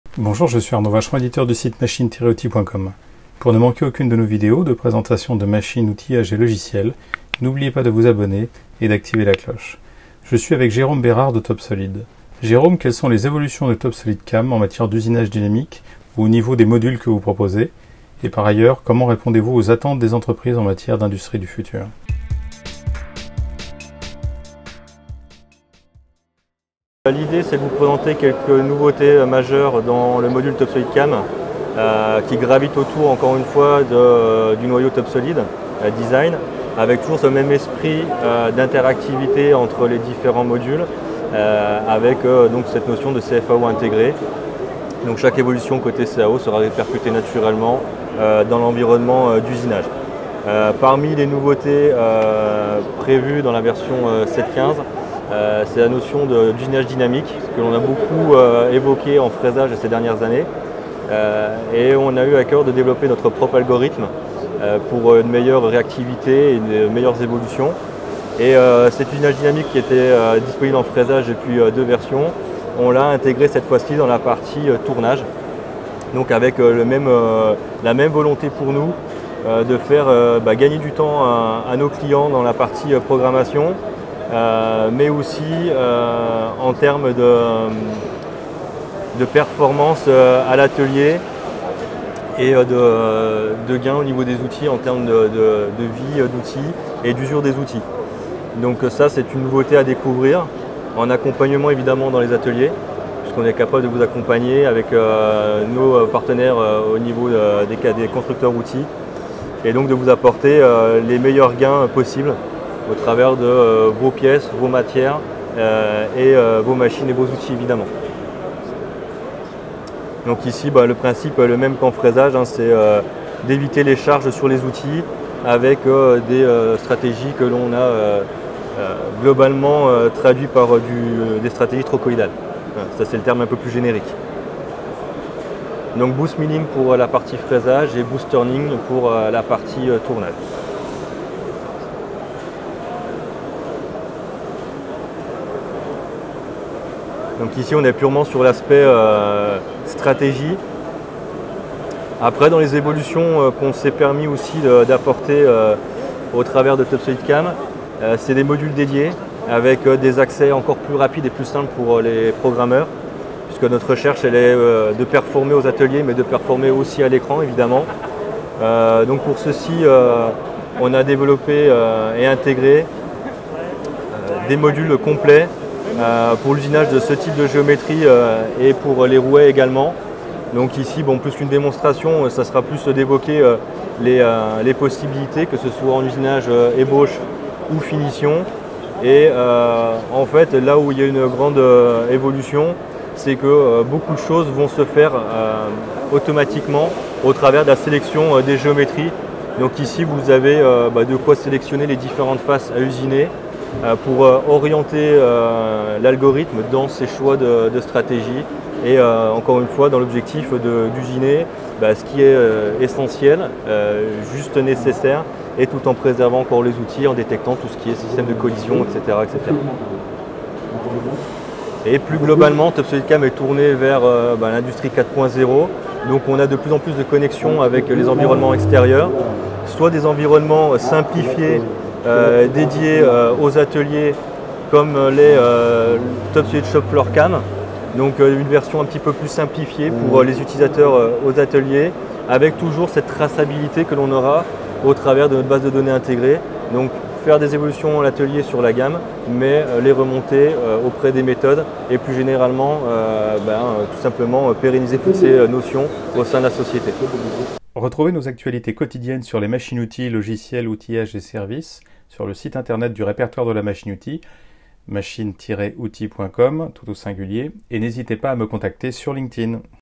Usinage dynamique trochoïdal en fraisage et tournage sur TopSolid'Cam
*** Plus d'Actualites machines, Logiciels, Outillages:
Interview de Jérôme BERARD de TOPSOLID.

La nouvelle version de TopSolid'Cam intègre l'usinage dynamique tant en tournage qu'en fraisage. Tous les modules de la suite CFAO ont accès à une base de données commune, qui permet de répercuter les modifications au niveau de la CAO dans la FAO usinage.